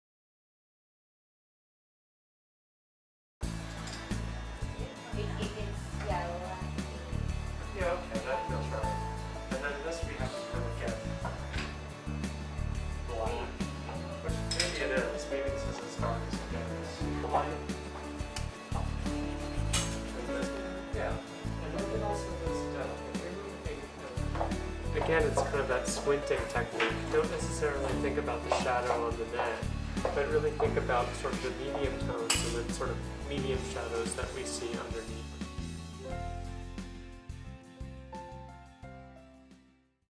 92Y Drawing Class
The stronger your drawing skills are, the better your paintings will be. Using pencil and charcoal, learn to draw from direct observation. Work mostly from still life, but get an introduction to the figure as well.